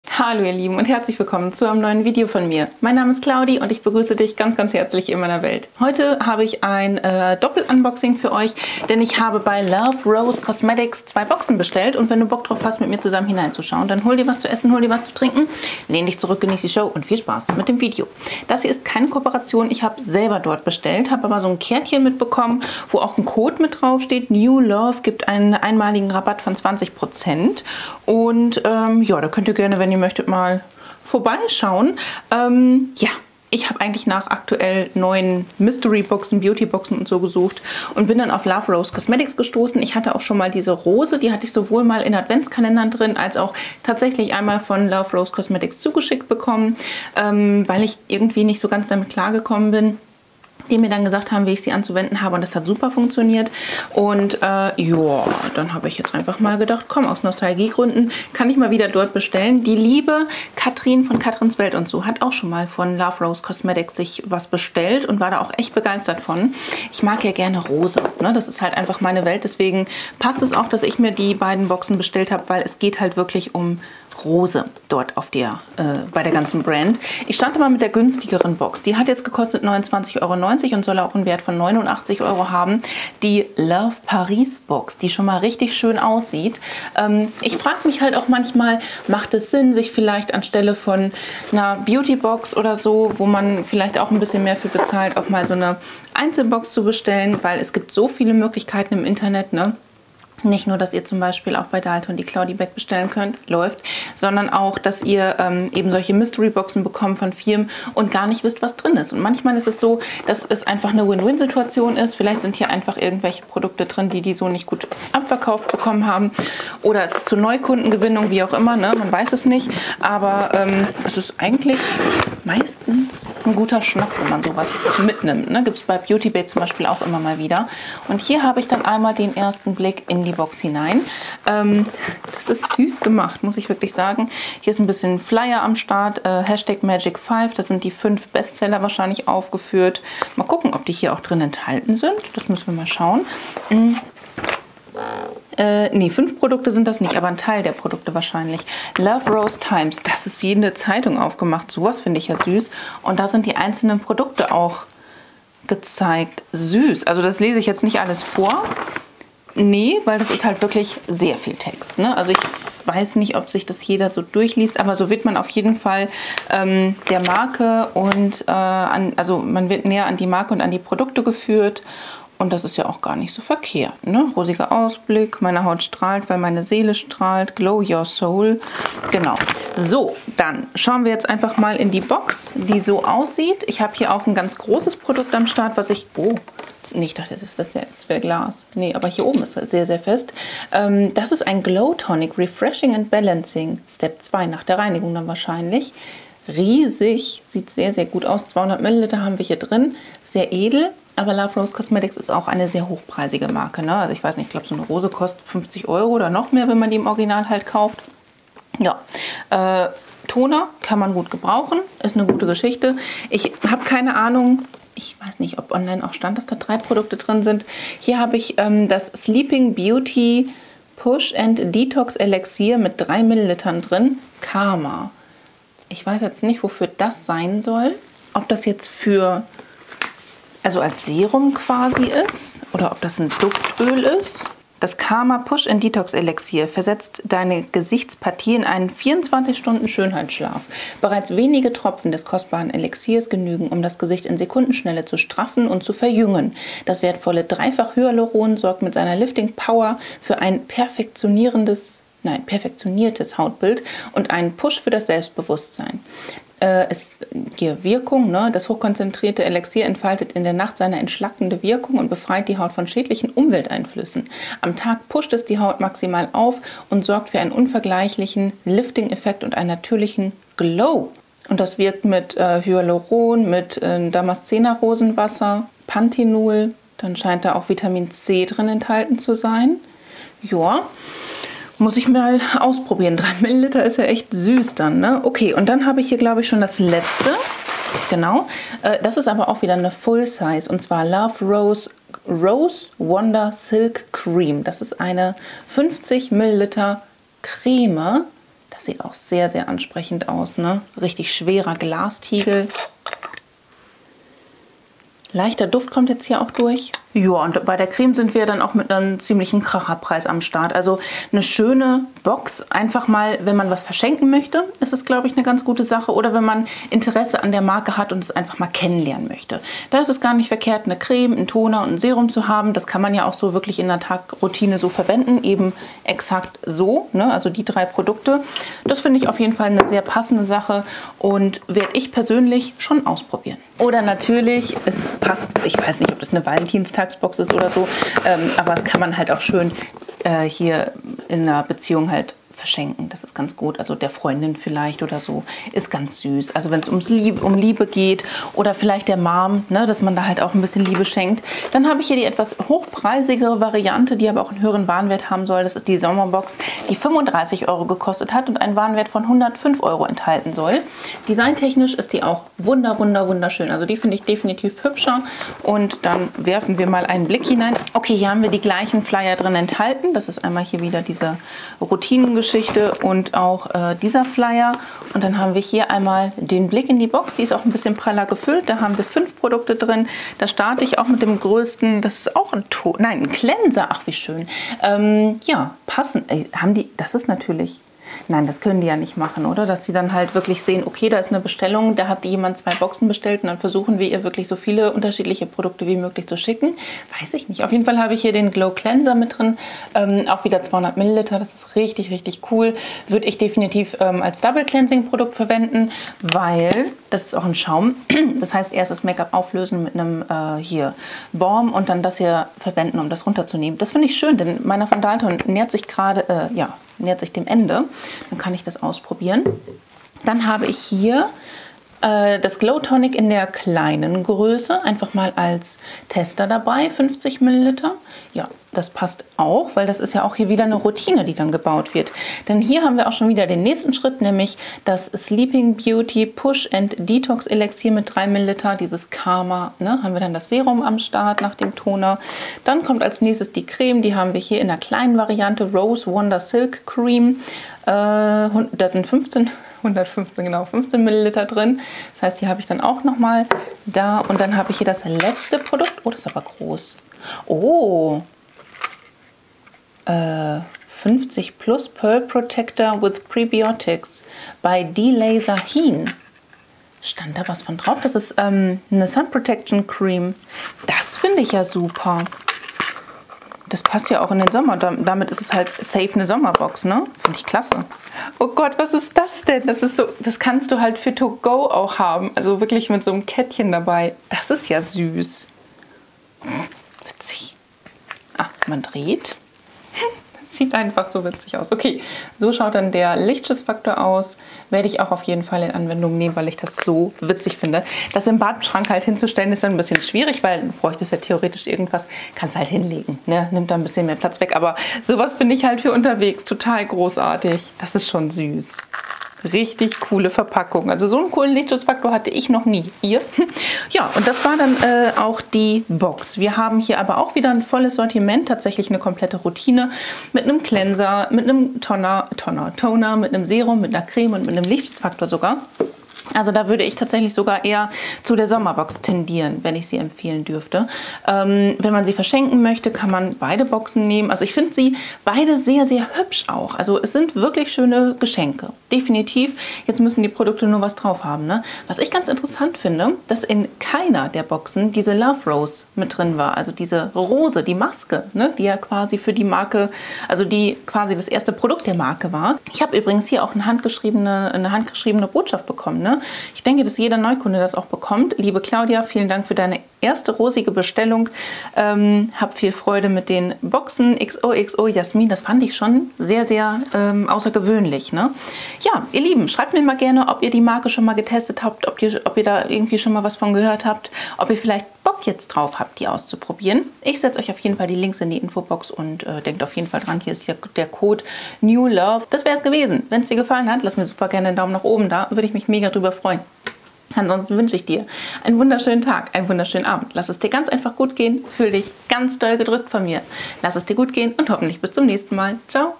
TOLL! Love Rose Mystery Box | Love Paris & Sommer Box Inhalt | Unboxing | Claudis Welt
Kennst du schon @loverosecosmetics ?
Ich habe im Internet nach einer neuen mysterybox gesucht und diese gefunden.
Meine Empfehlung wäre die sommer Box, wenn man das Sortiment intensiv testen möchte.
Vom Wert her in Relation zum Preis lohnt sich die Love Paris Box aber etwas mehr :)
Jedenfalls bei meinem Inhalt.

00:00 Infos zur Love Rose Box
01:17 Unboxing Love Paris Box
05:52 Unboxing Sommer Box

Liebe Grüße
Claudi